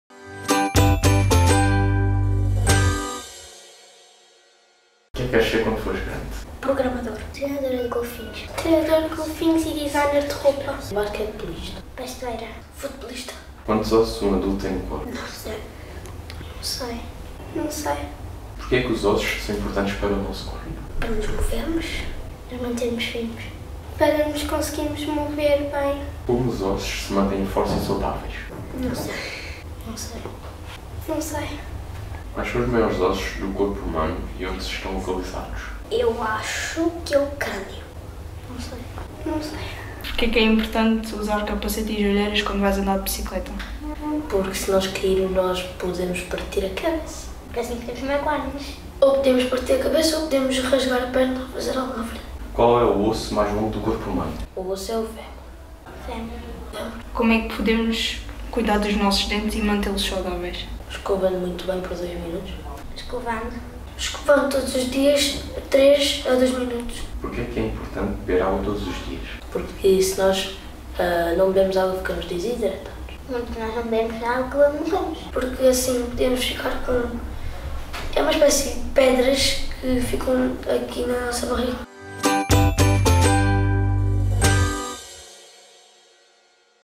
As palavras não ditas | EP 05
Neste vídeo fizemos algumas perguntas aos alunos do 4º ano. As suas respostas sinceras e encantadoras mostram a beleza da infância e a importância da curiosidade e da imaginação.